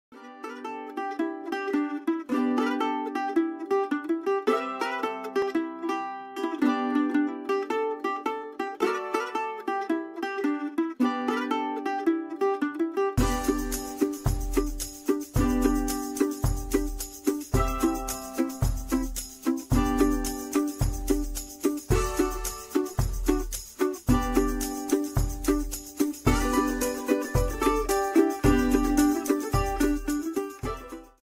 Kiki the Ferret's Favourite Foods
Kiki is eating some of her favourite foods. Watch this video to find out what the foods are!